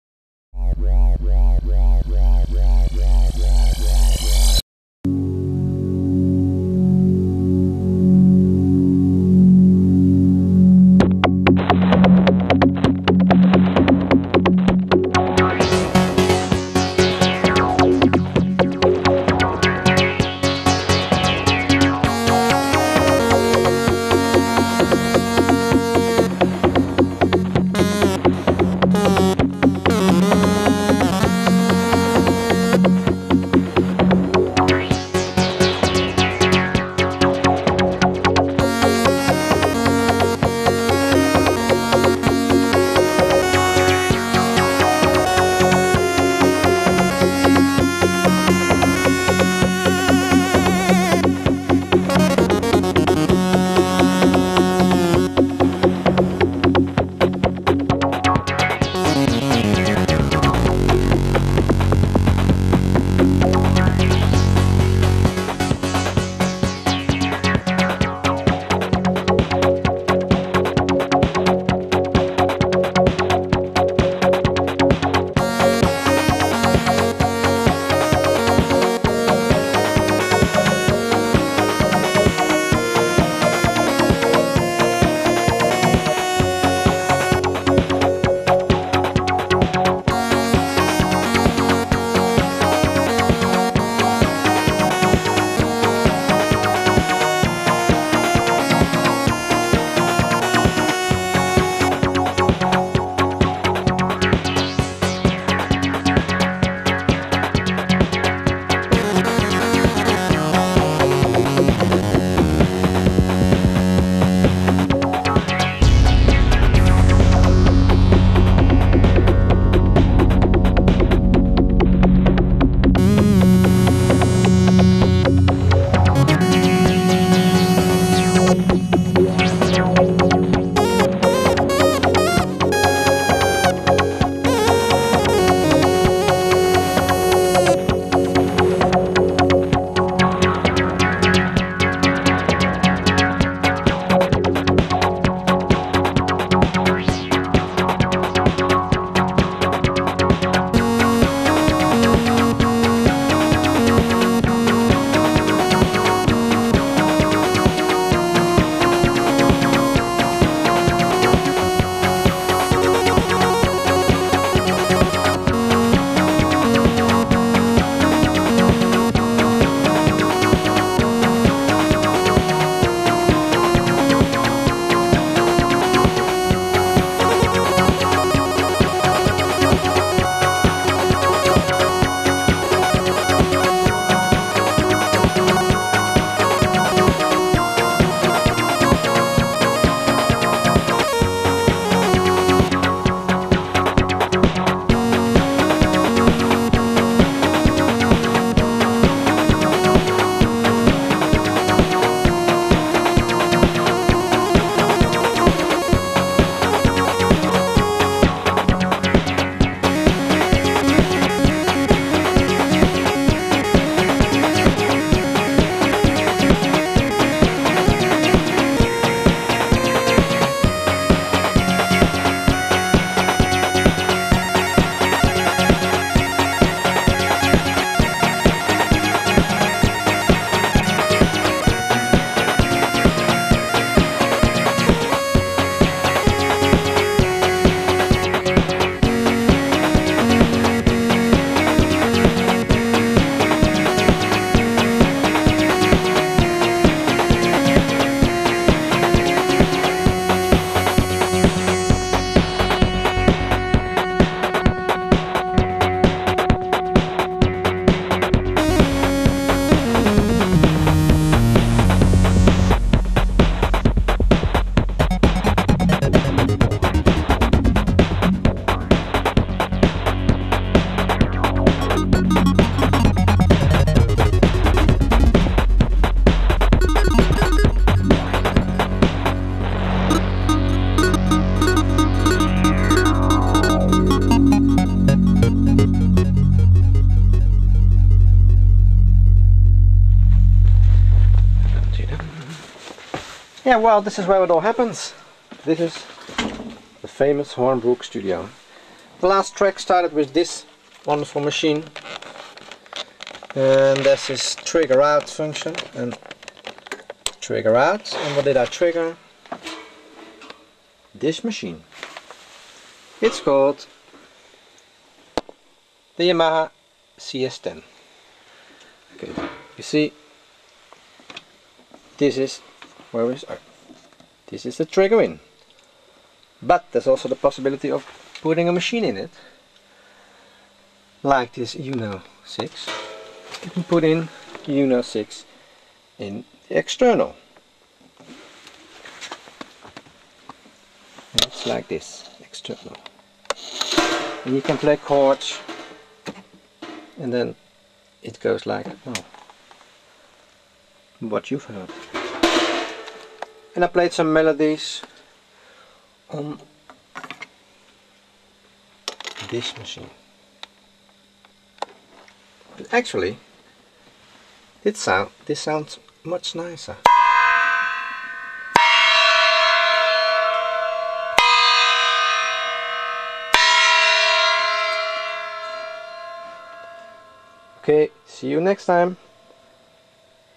Yamaha CS-10 external in
The Dr-220 drumcomputer triggers the Yamaha CS-10 ( trigger in /trigger out). The Roland Juno 6 is connected to the CS-10 via the External In . At the end of this clip I explain everythng. Enjoy !